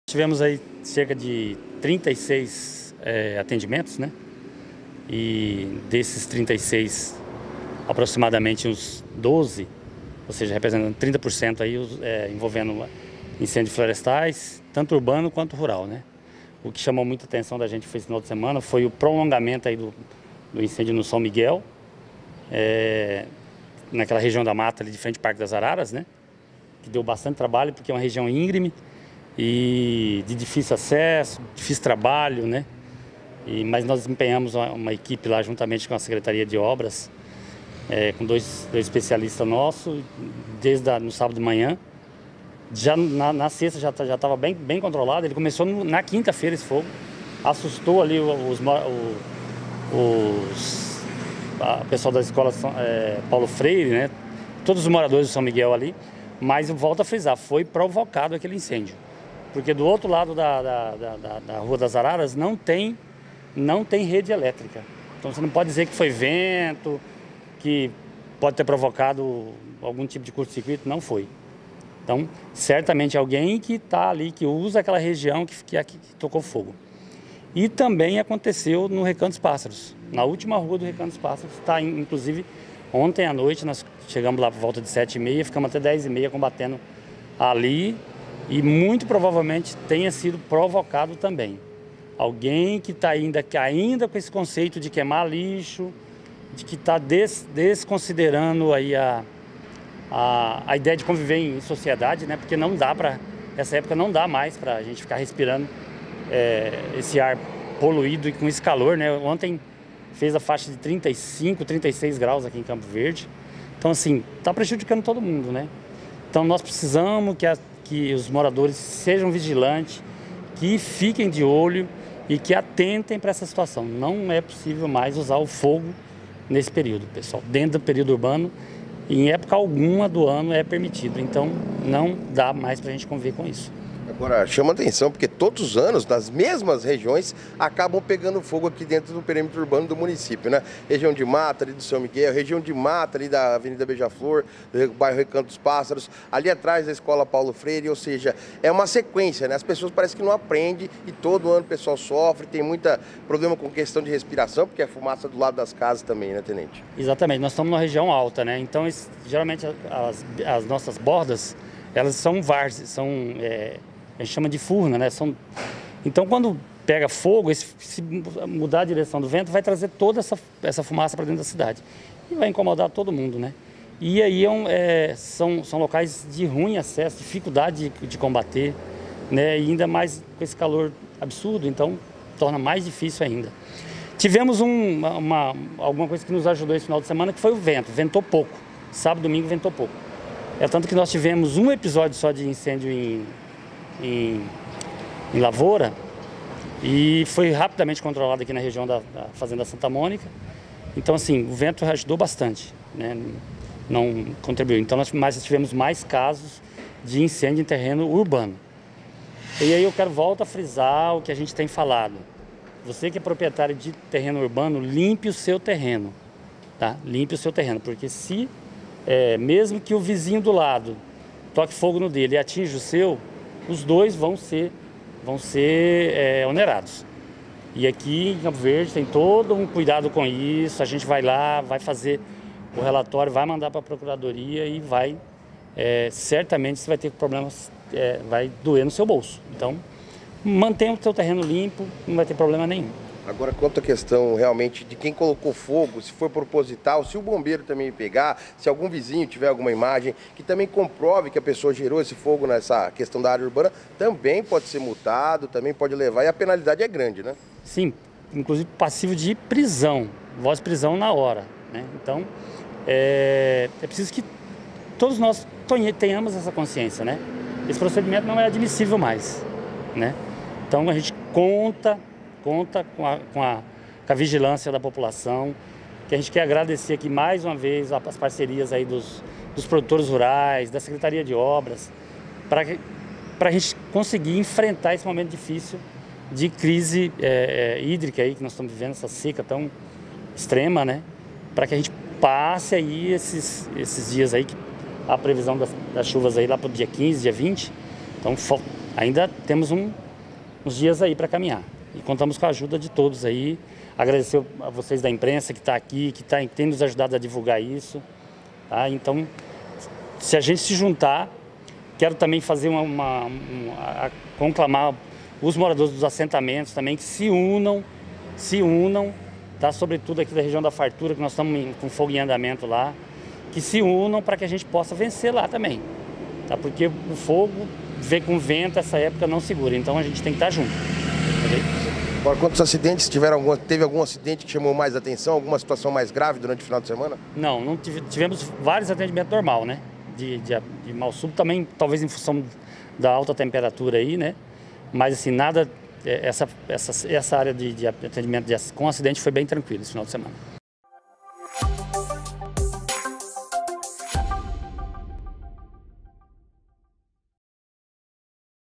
Corpo de Bombeiros registram incêndi0s no final de semana em Campo Verde
Tv Real - SBT  7.1 HD
Campo Verde - Mato Grosso